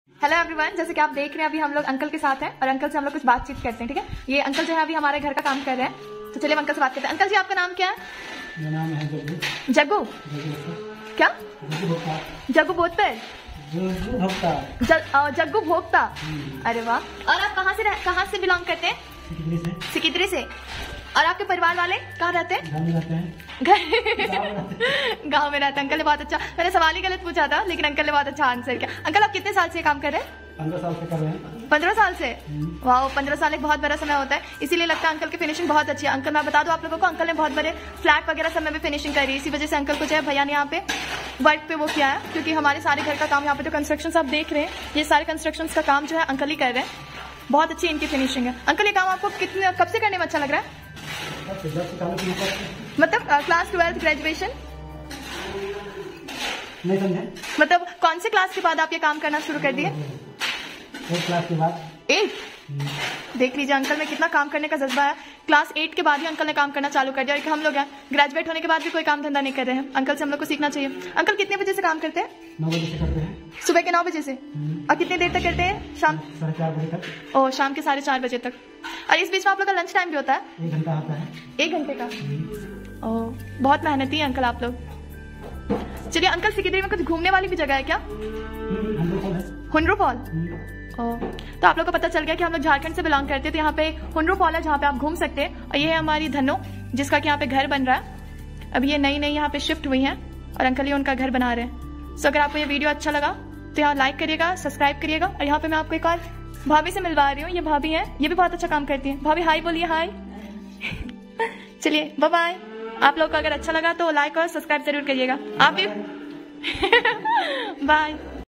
"खेतों से महलों तक || From Fields to Foundations: Interviews with Rural Construction Workers 💪🌿
Join us as we venture into the lives of rural laborers in India who have dedicated themselves to construction work. In this video, we engage in heartfelt conversations with them, listening to their challenges and successes. These are the unsung heroes who have toiled relentlessly to strengthen every structure. Through this video, you'll get a glimpse into their fascinating stories. Witnessing these invaluable experiences will leave you with a deep appreciation for the exceptional and hardworking human resources in India's rural areas."